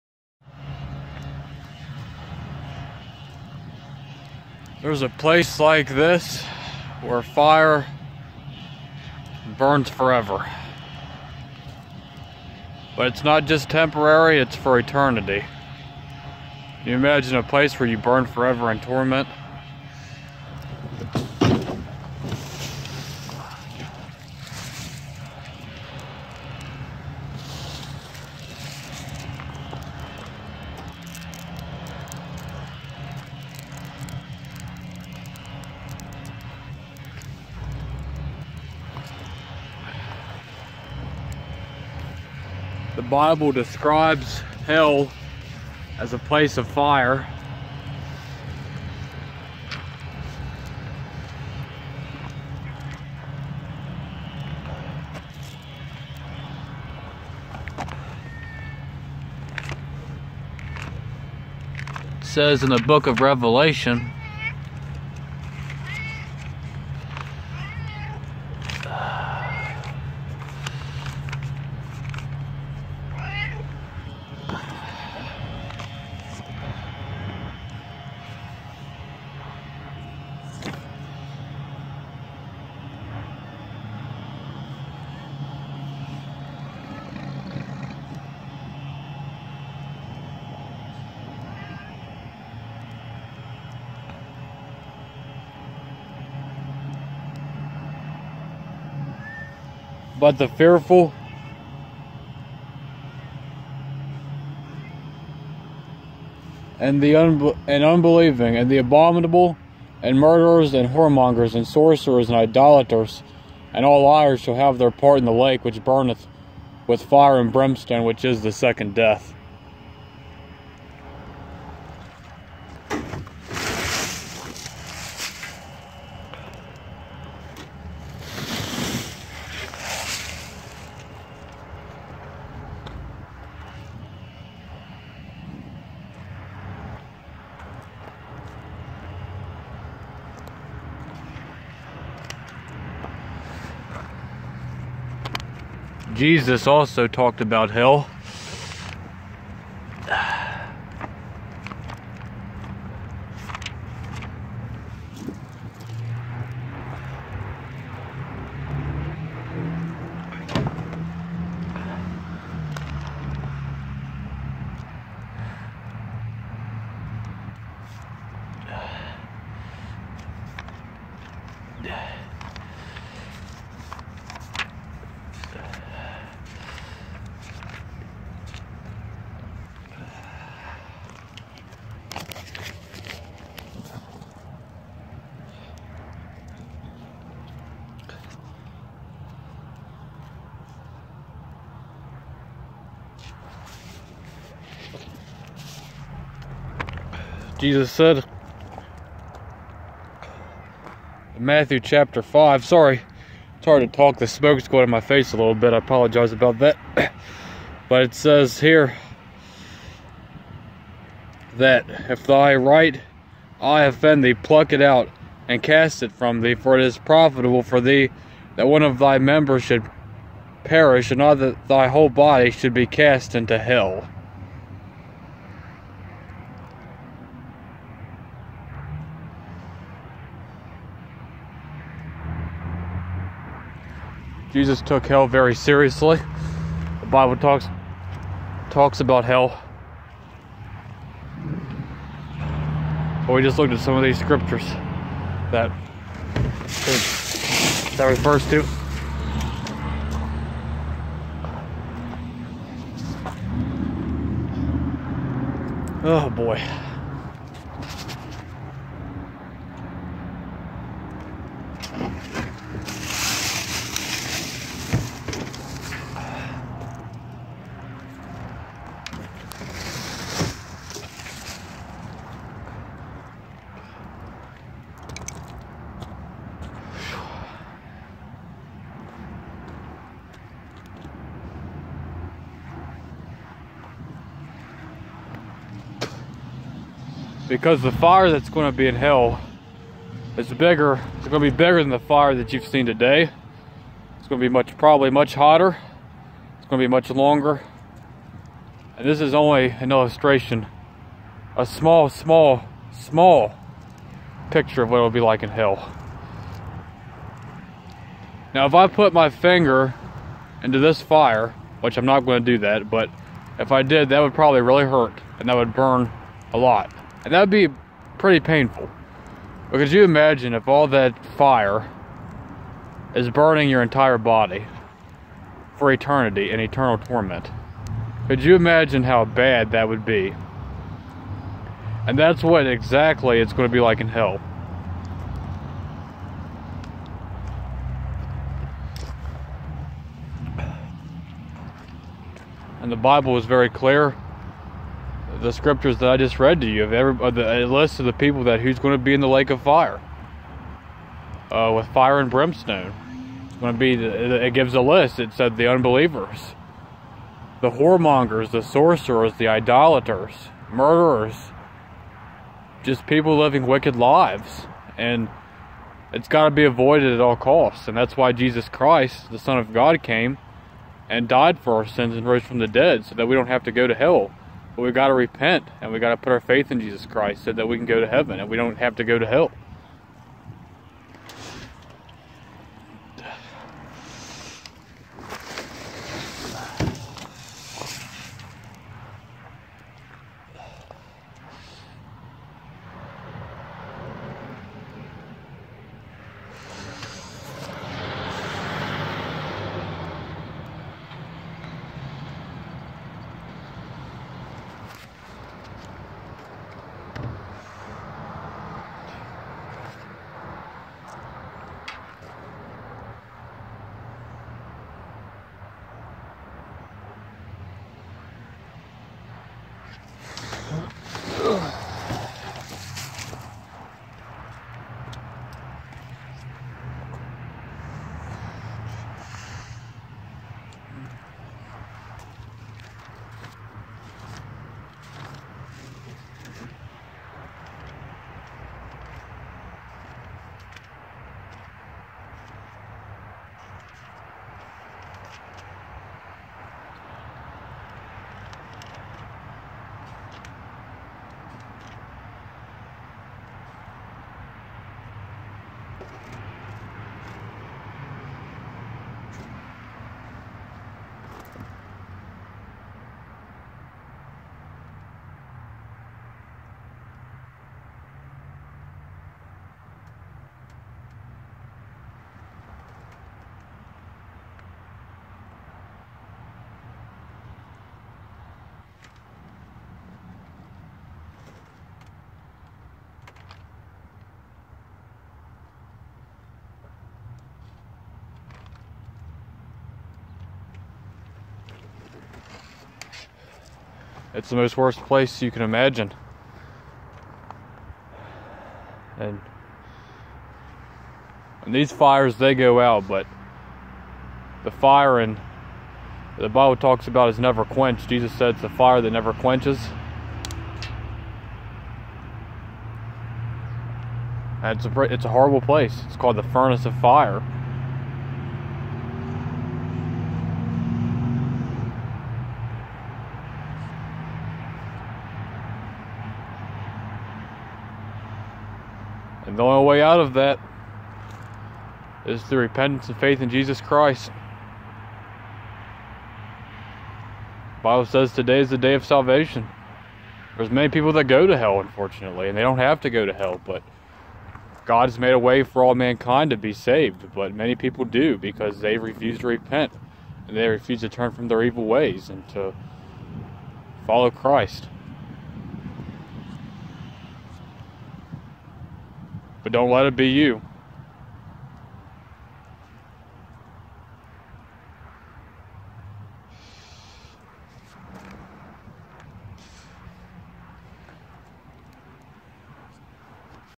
An Illustration of Hell
This is a video I made while burning some leaves that gives an example of hell. I hope this will challenge you and get you to consider